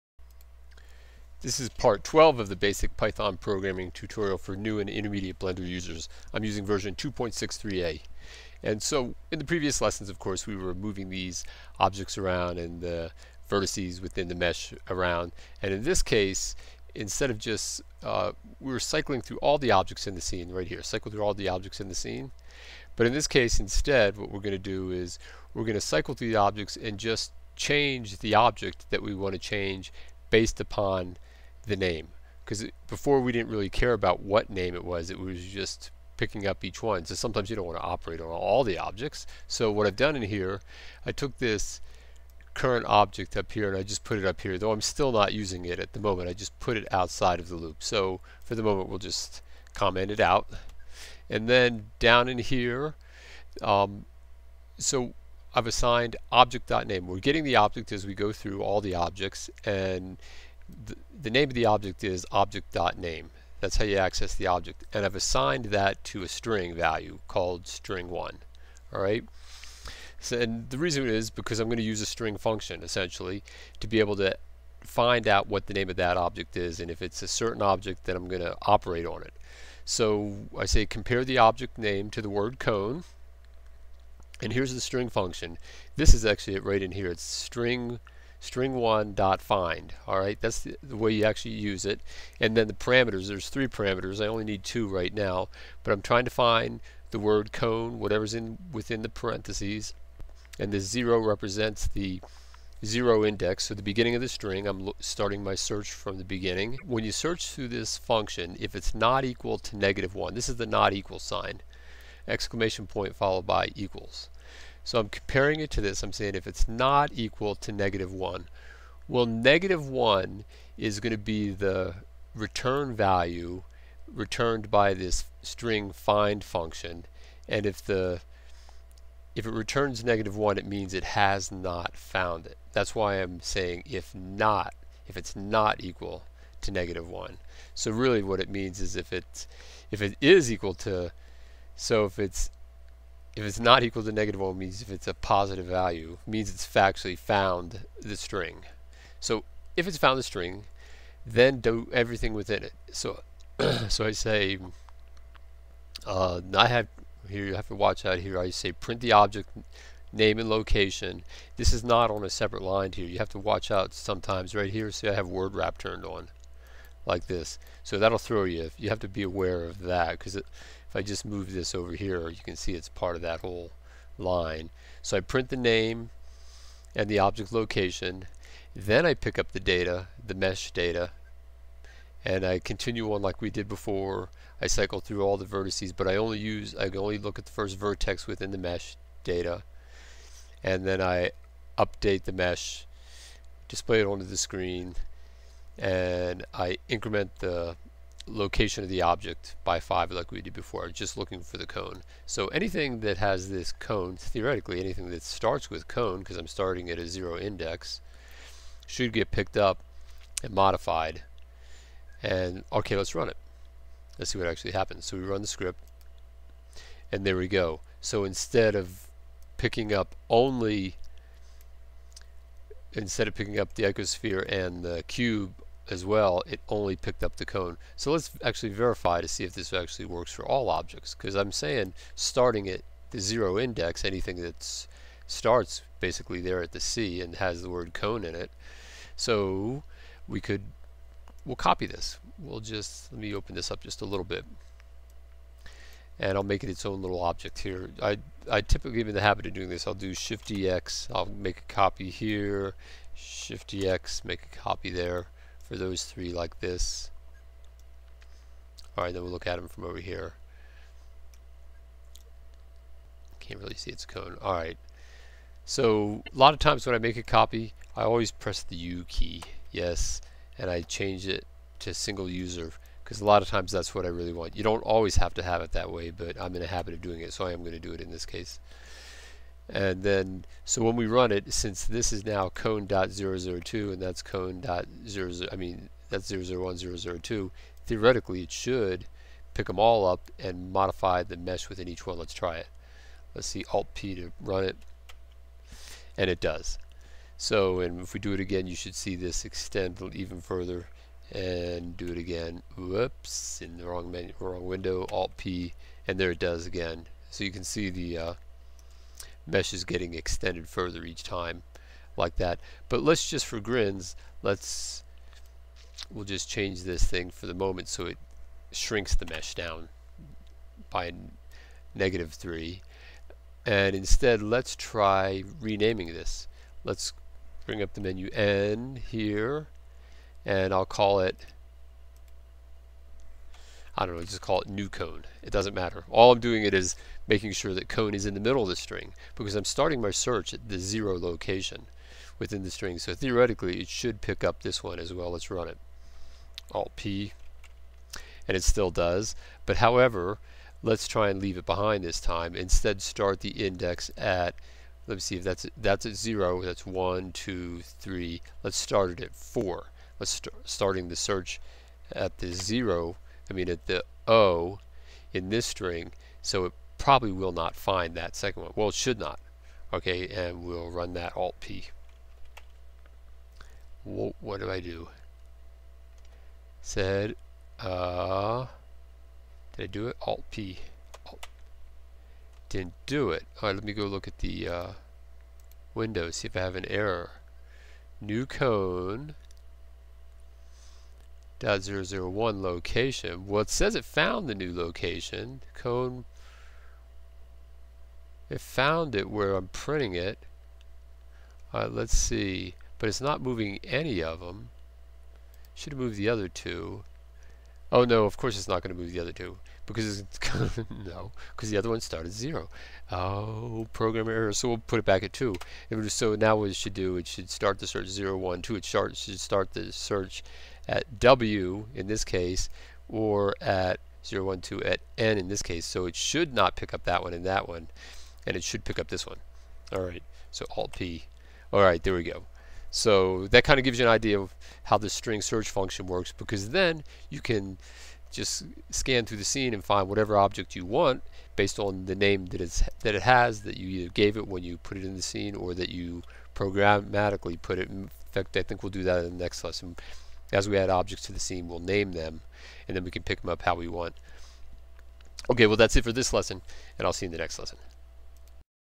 Blender Python Tutorial - Basic Python Programming - Part 12
Part twelve of my basic Blender Python Programming tutorial for new and intermediate Blender users. In this lesson, we look at how to search for any object in the scene by name.  This will be of extreme value in a couple of lessons from now as we'll be ready to easily control the attributes of each Vegas-style water fountain, once we get past these important programming details.